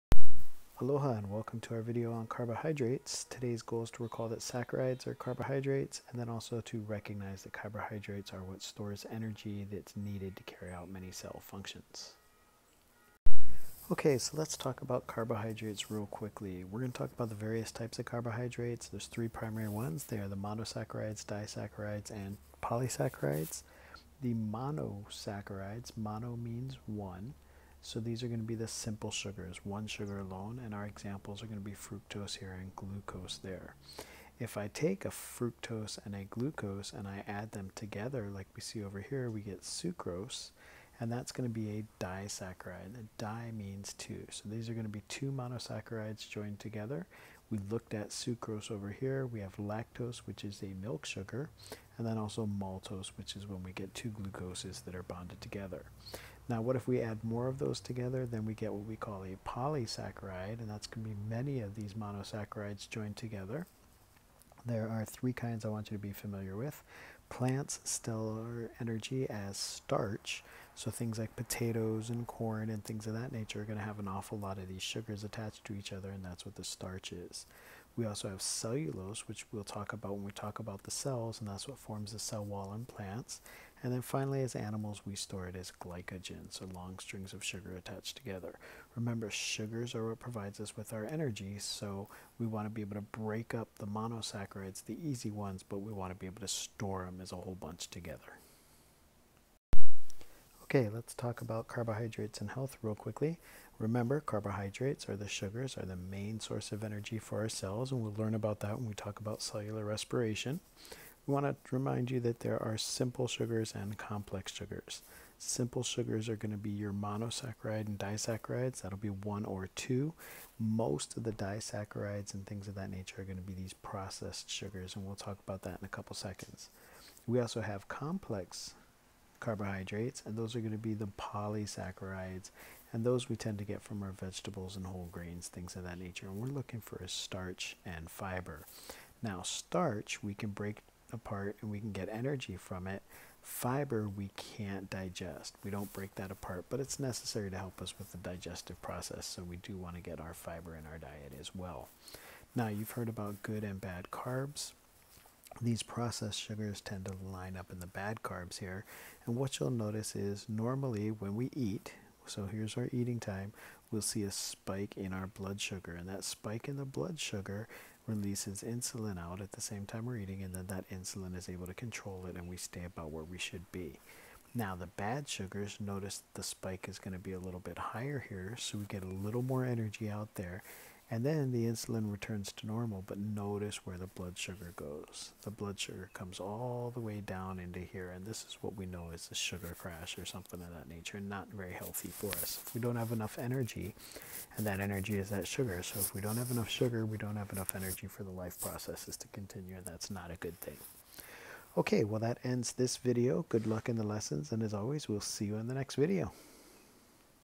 Biology A 2b.2a Carbohydrates
This is a short introductory video to carbohydrates.